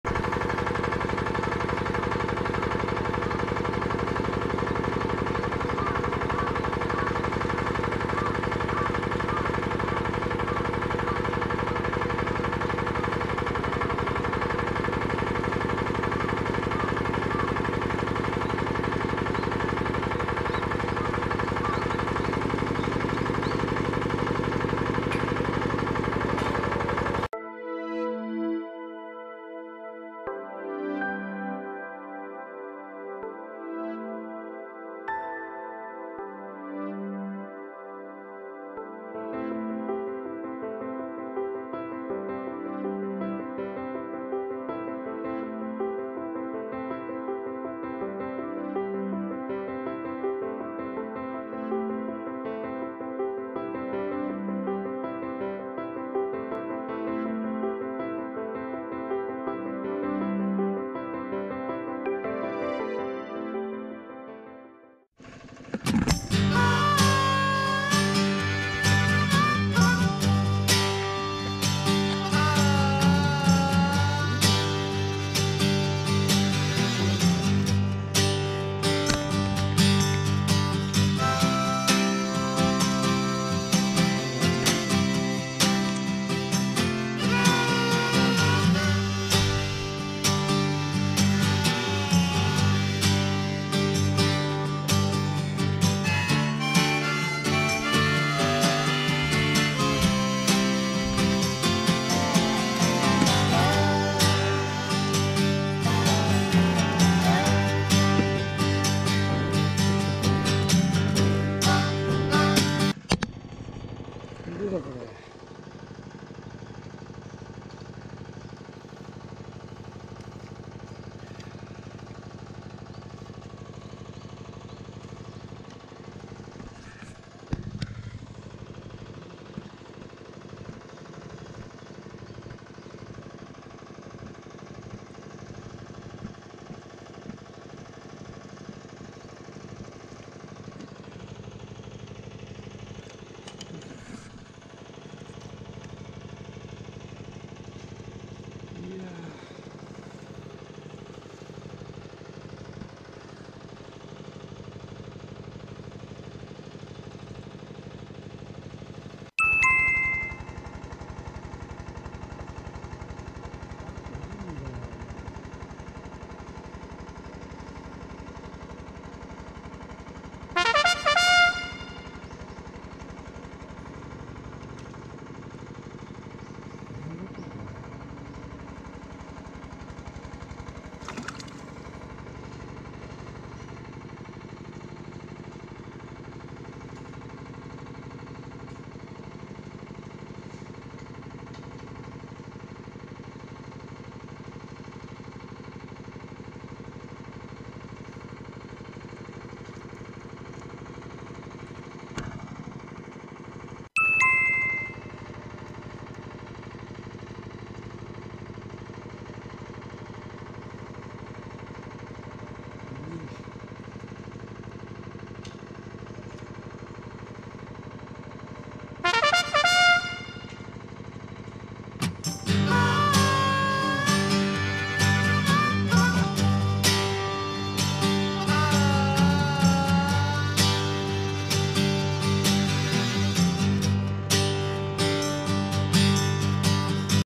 ダダダ 地質調査中の池で釣りをしてみた
地盤調査のためのチューブを打ち込んでいるなかでも魚は釣れるのか❔